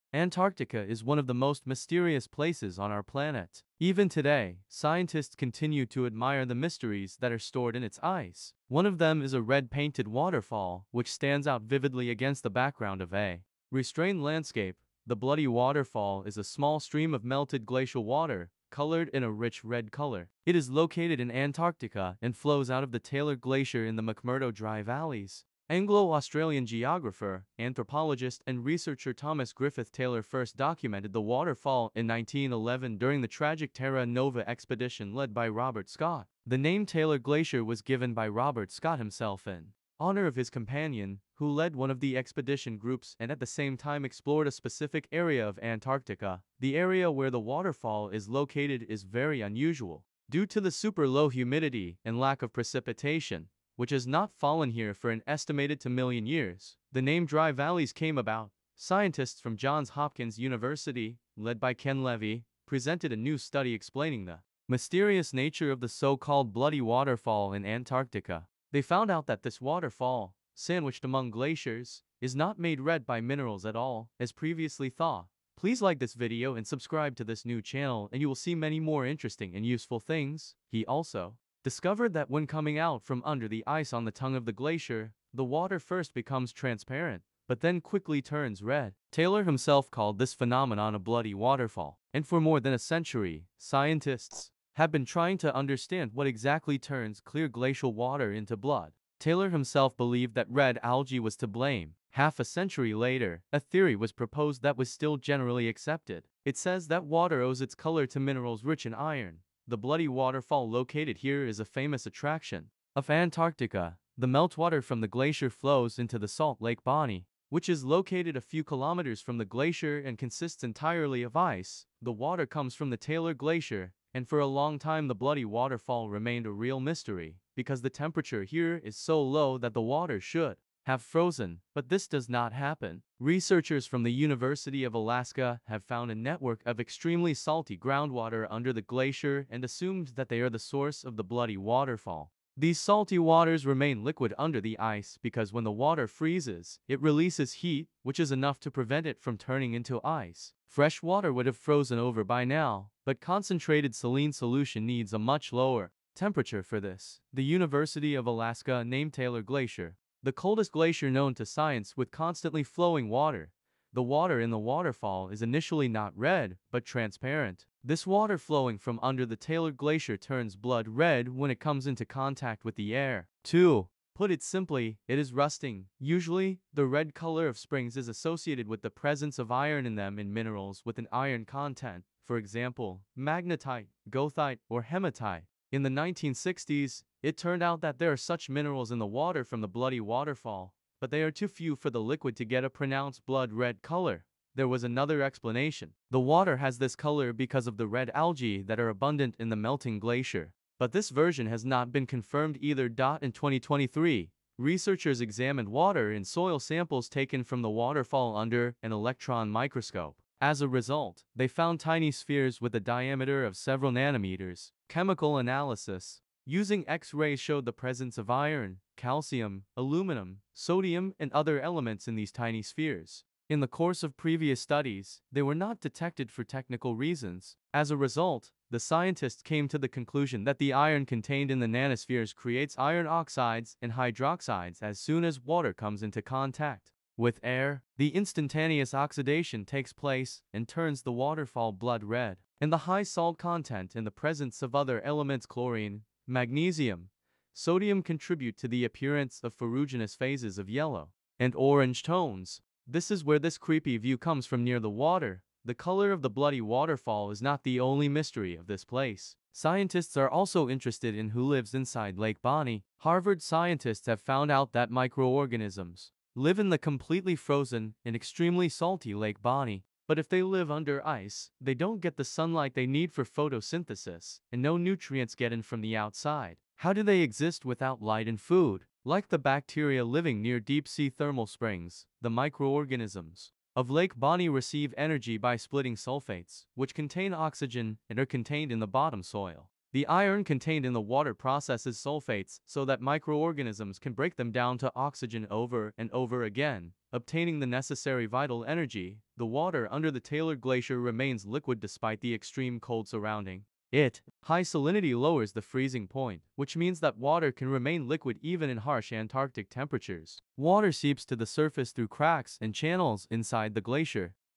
The mystery is solved
The Bloody Waterfall in Antarctica is a stream of salt water colored red due to its high iron oxide content. The water flowing out from under the glacier is rich in iron, which, when in contact with oxygen, oxidizes and acquires a blood-red hue.